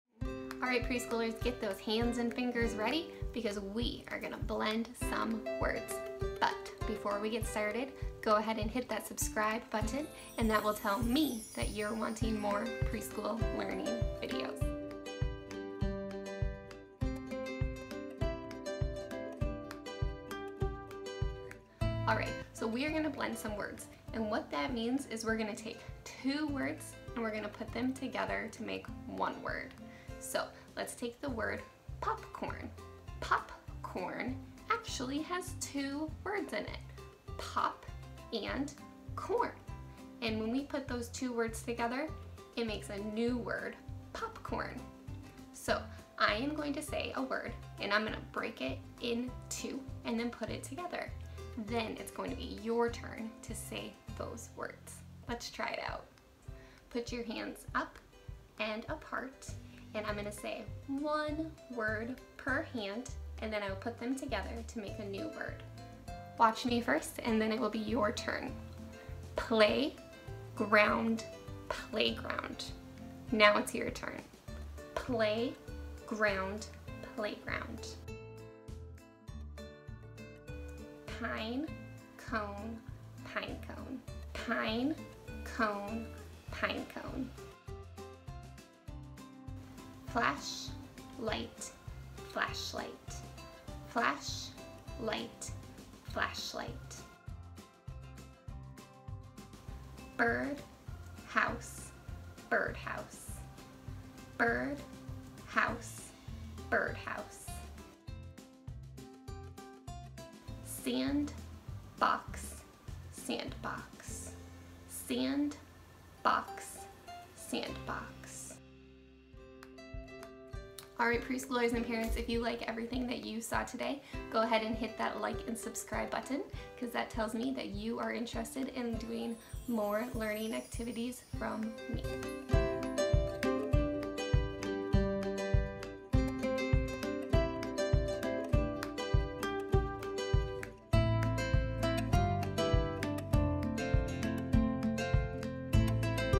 PRESCHOOL BLENDING ACTIVITY with COMPOUND words: The first step in the blending skill sequence
Hi, my name is Amber and in this video I am inviting your young learner to blend compound words with me. Compound words are when two or more words come together and create a new word that means something different. For example, "pop" and "corn" are two different words but when they come together they form a new word, popcorn! Blending compound words is the first step to teach in the blending sequence, followed by blending syllables and blending onset-rime into spoken words.

My preschool class loves blending words and participating in phonemic awareness activities. I hope that your young learners enjoy blending just as much!